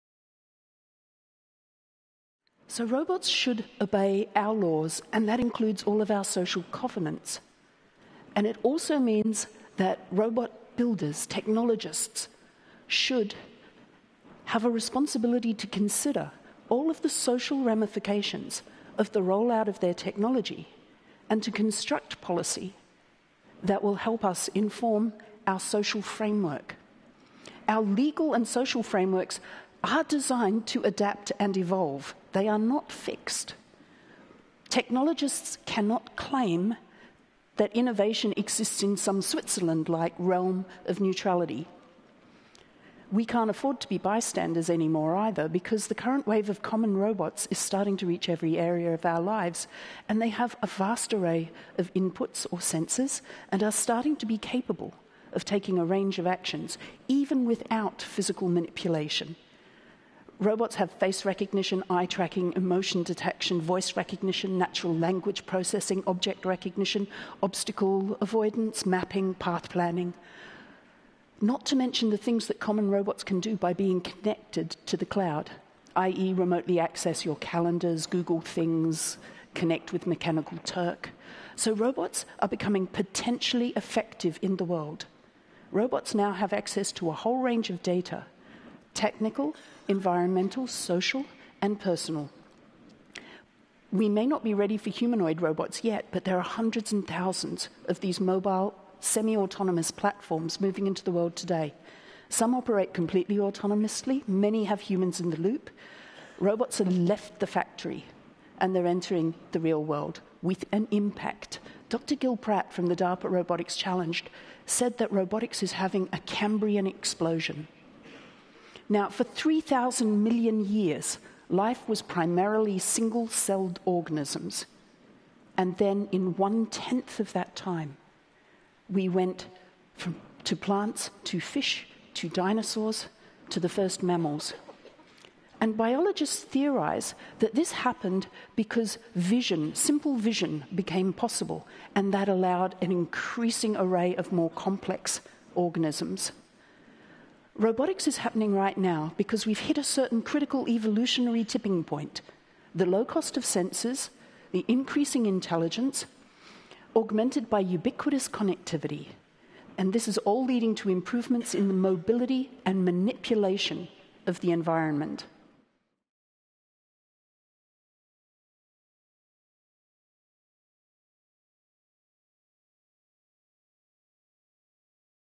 Making good robots Andra Keay (Silicon Valley Robotics)
Let’s stop talking about bad robots and start talking about what makes a robot good. A good or ethical robot must be carefully designed. Andra Keay outlines five principles of good robot design and discusses the implications of implicit bias in our robots. We are encountering more and more robots in our lives, so we designers need to be creating good interactions or else we may wind up in a world of robot smog, robot spam, and robot stereotypes.